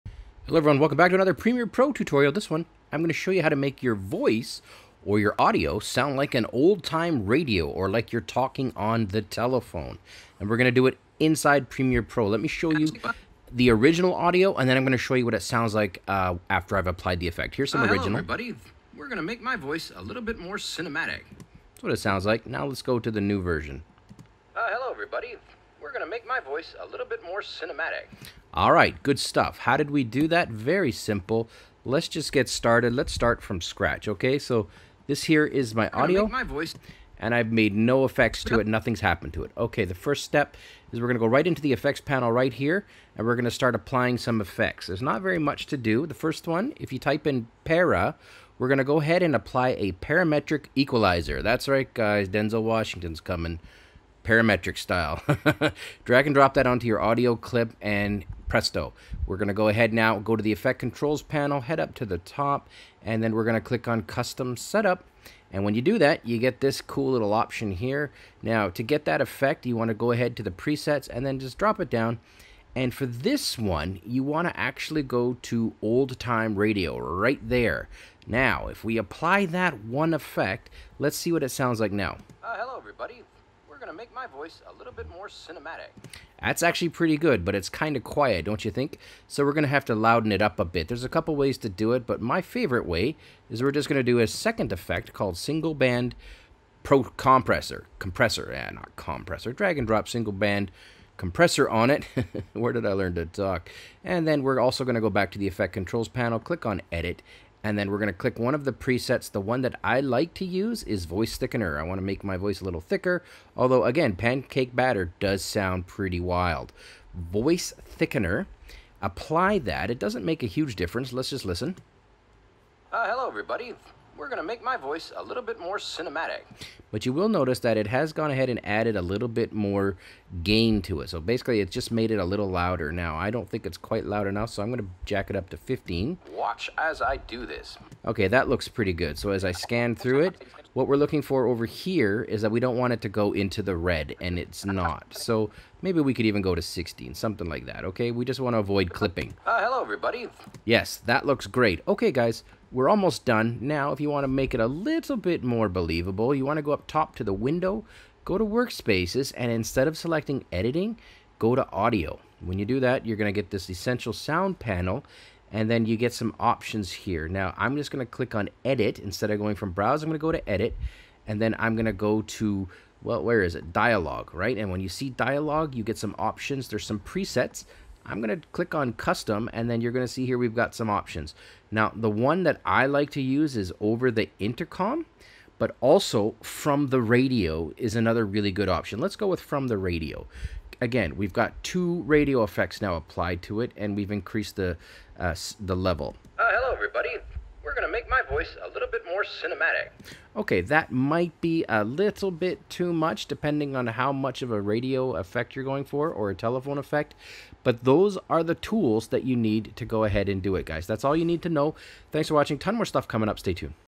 How To Create A Telephone Effect / Old Time Radio Effect - Premiere Pro
Creating an old time radio effect or a telephone audio effect is easy to do in adobe premiere pro.  We use three separate effects - the parametric equalizer, the single band processor, and the From the Radio effect in the essential sound panel.